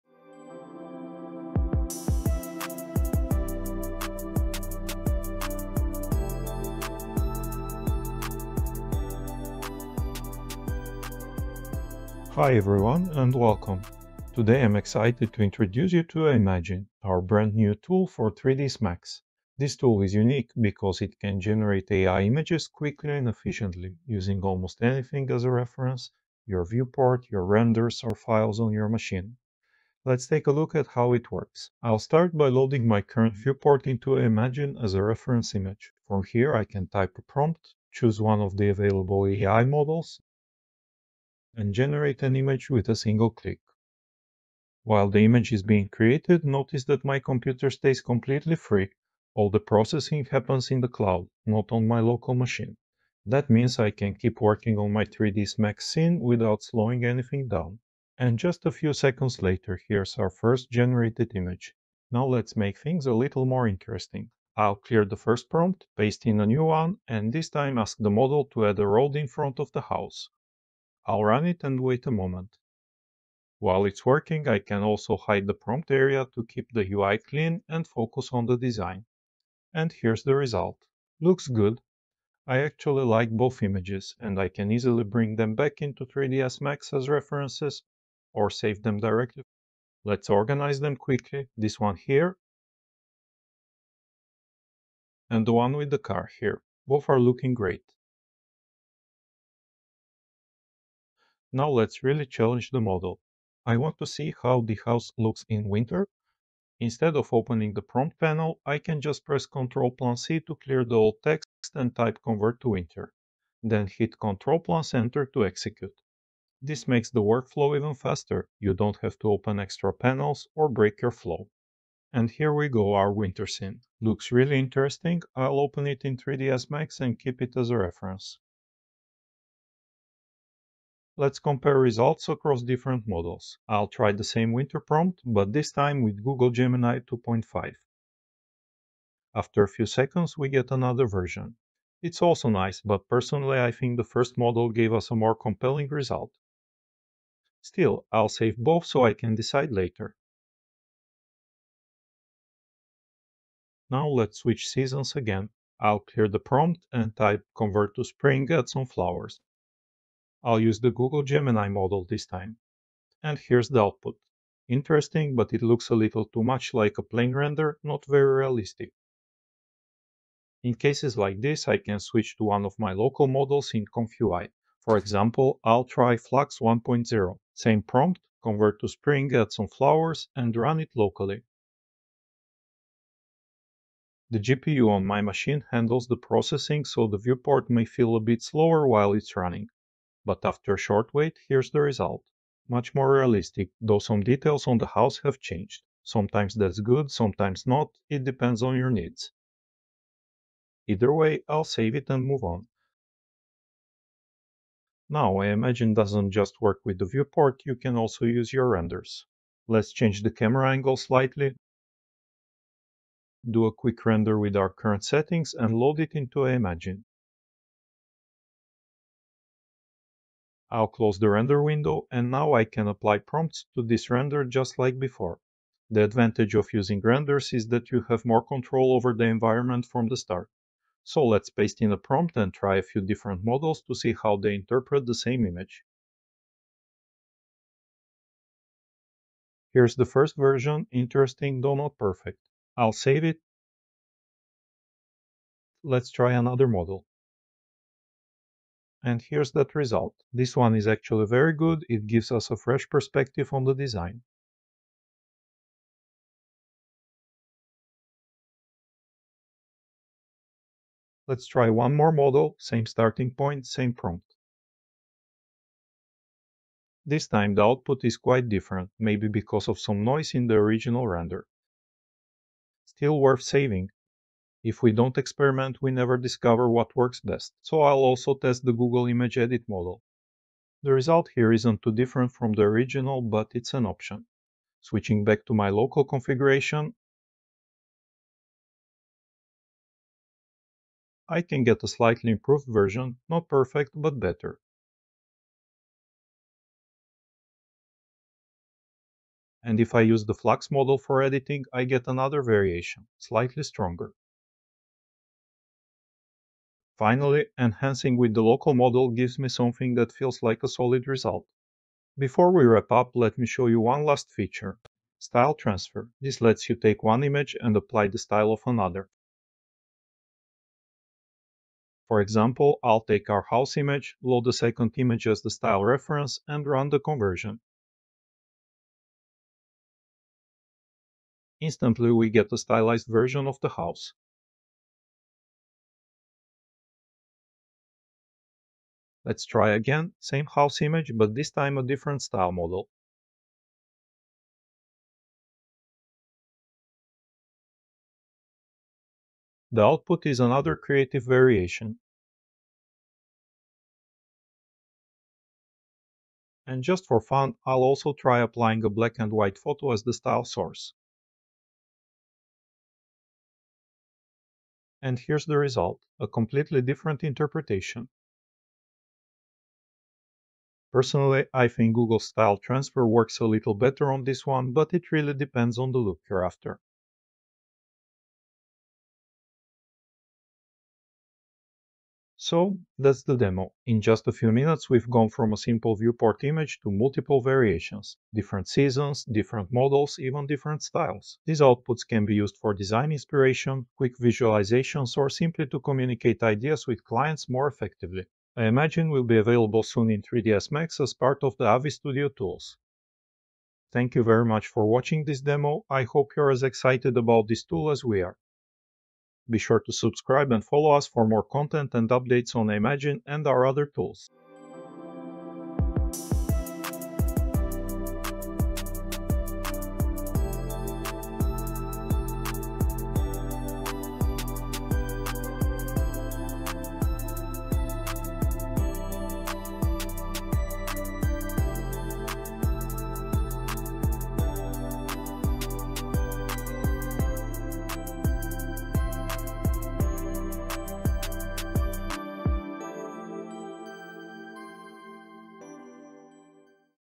AI Image Generation Inside 3ds Max - Introducing AImagine
Meet AImagine, our brand-new AI-powered tool for 3ds Max that brings the latest and most advanced image generation models directly into your workflow.

With AImagine, you can:
✨ Generate images instantly from your viewport, renders, or files
🌦 Transform your scenes across different seasons – summer, winter, spring
🚗 Add details and new elements to enrich your designs
🎨 Apply style transfer to explore creative and artistic variations
⚡ Compare outputs between cloud-based models and local GPU models for the best balance of speed, cost, and quality

Designed for architects, designers, and 3D artists, AImagine helps you explore concepts faster, present better visuals, and communicate ideas with clients like never before.

🚀 Powered by the latest and best AI image generation models available: Nano Banana: Google's Art Tool, OpenAI GPT Image 1 Edit, , FLUX Kontext Pro, Seedream-4 Image Edit
🔔 AImagine will be released soon as part of Aviz Studio Tools for 3ds Max.